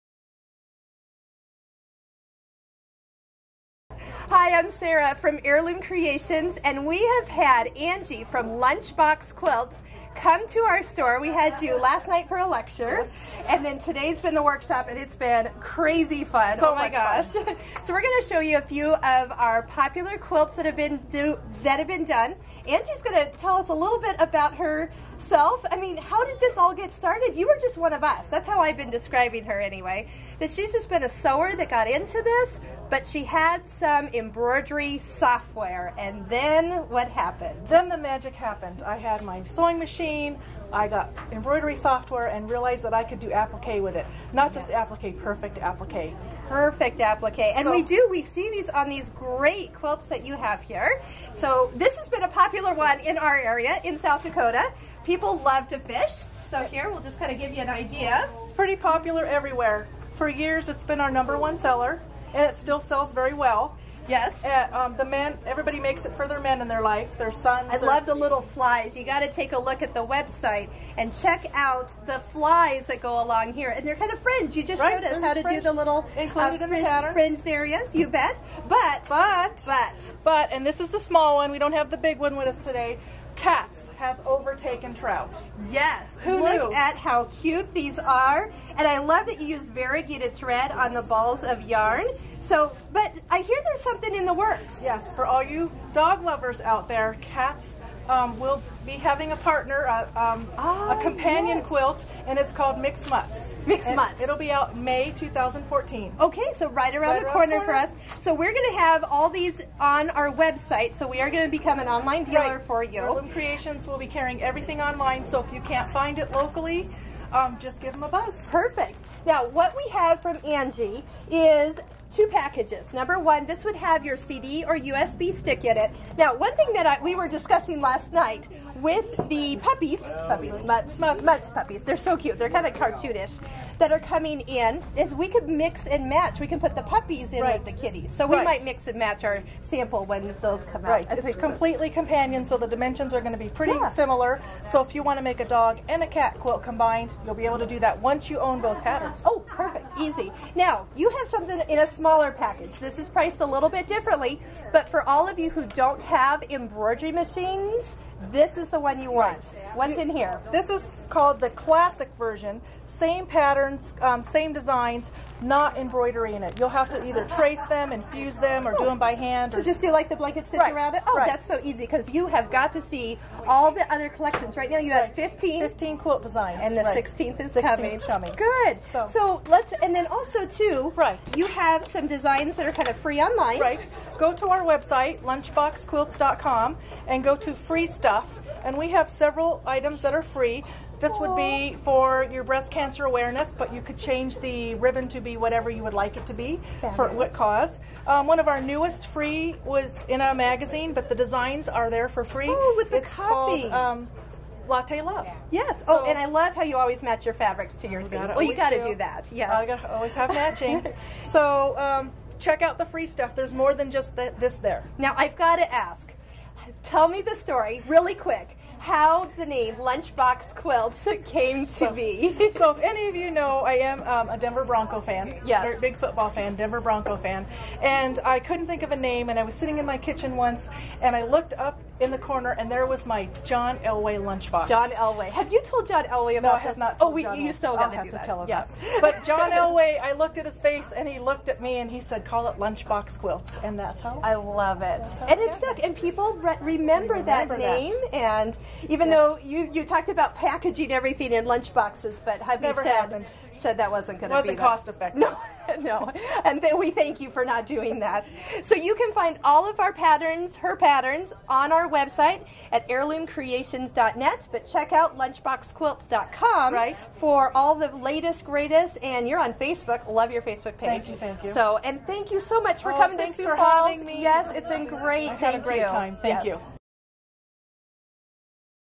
Lunch Box Quilts and John Elway?
We recently had the pleasure of hosting an embroidery event with Lunch Box Quilt's Angie Steveson. She was kind enough to take some time to answer a few questions.

Heirloom Creations
3800 S Western Ave
Sioux Falls SD 57105